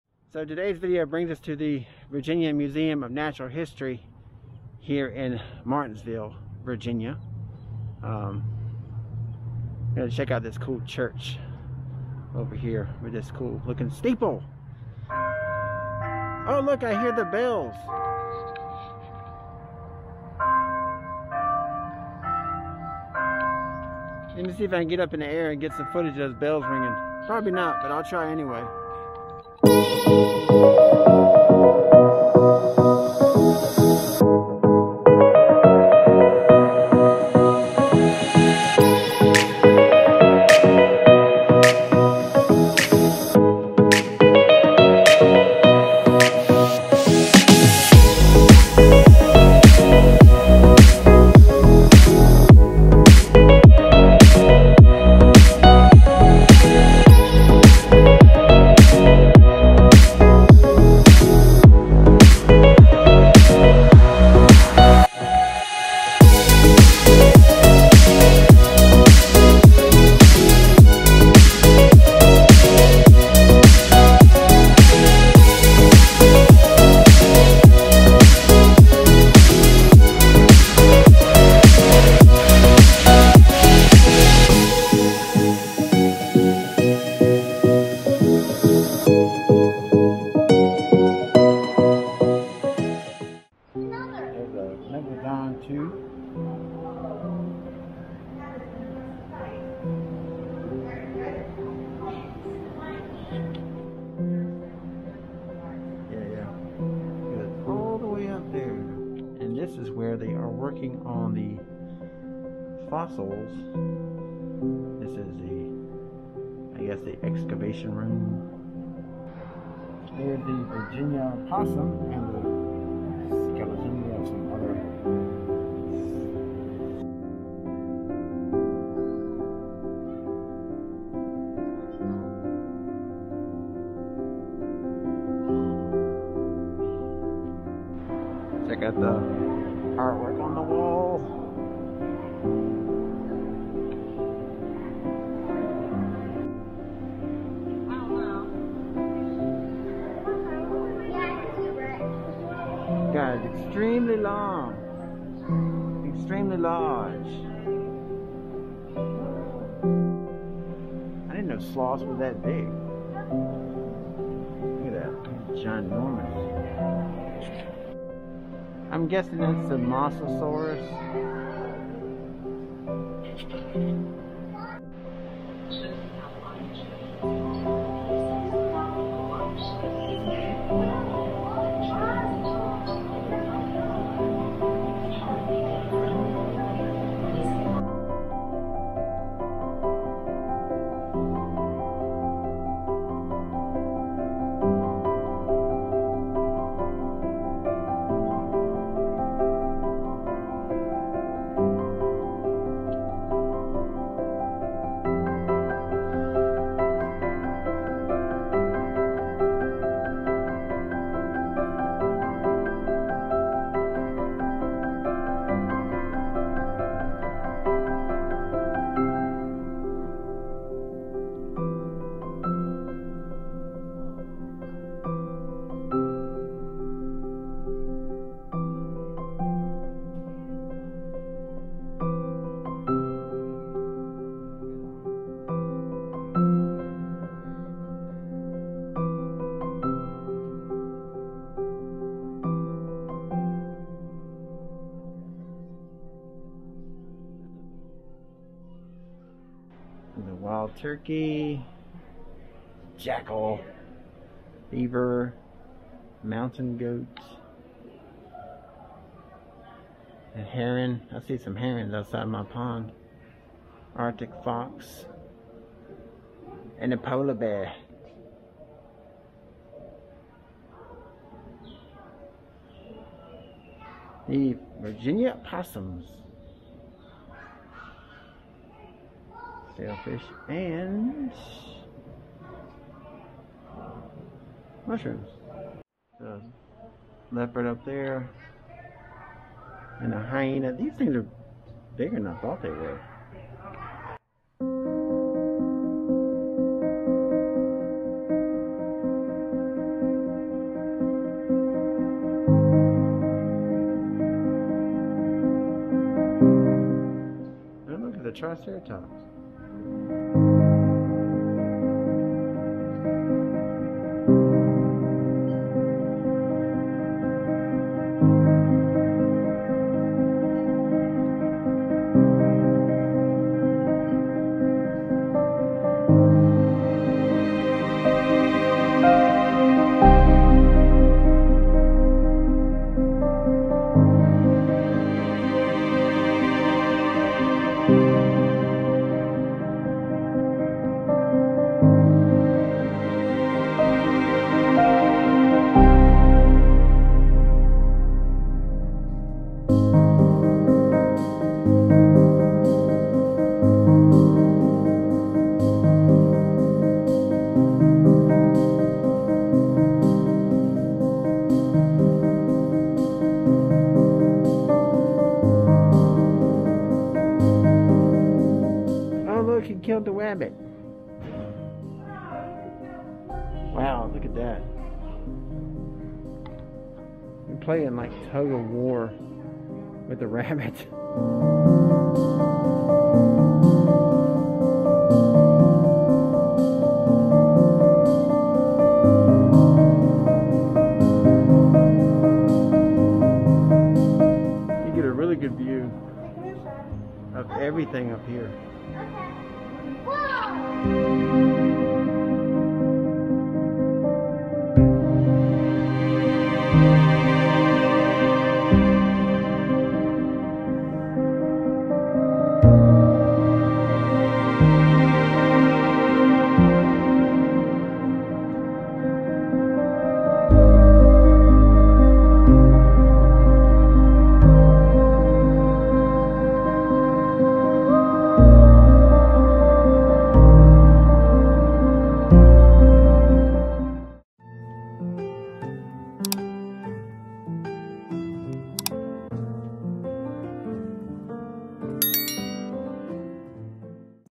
Virginia Museum Of Natural History (MartinsVille Virginia)
Took a quick trip to MartinsVille Virginia to check out this museum.
DJI Osmo Action & Mini 2